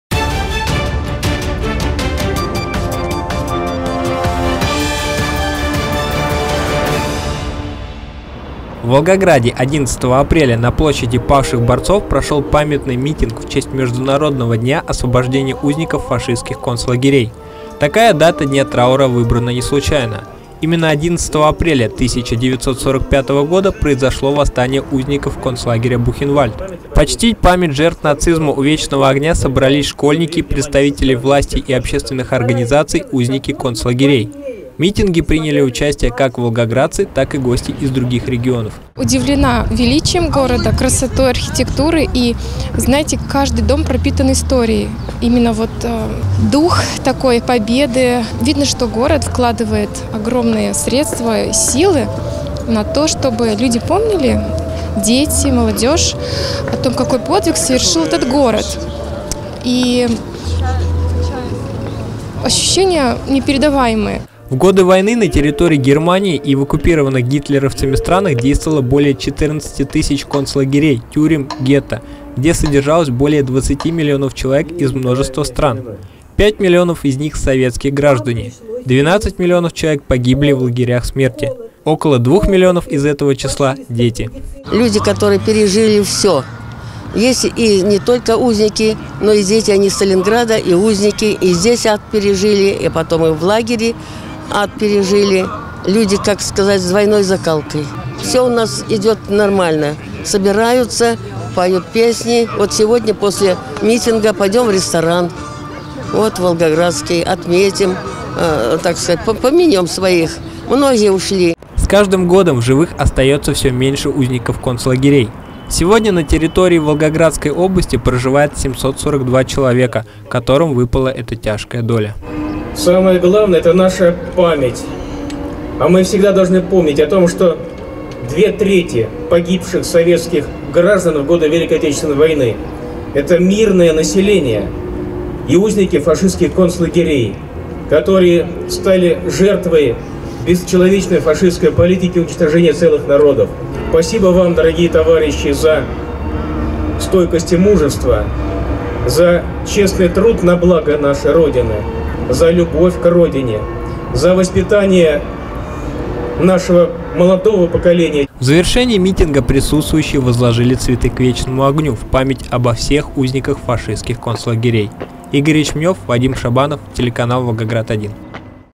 В Волгограде прошел митинг в честь Международного дня освобождения узников фашистских концлагерей
В Волгограде 11 апреля на площади Павших Борцов прошел памятный митинг в честь Международного дня освобождения узников фашистских концлагерей. Такая дата Дня траура выбрана неслучайно. Именно 11 апреля 1945 года произошло восстание узников концлагеря Бухенвальд. Почтить память жертв нацизма у Вечного огня собрались школьники, представители власти и общественных организаций, узники концлагерей. В митинге приняли участие как волгоградцы, так и гости из других регионов.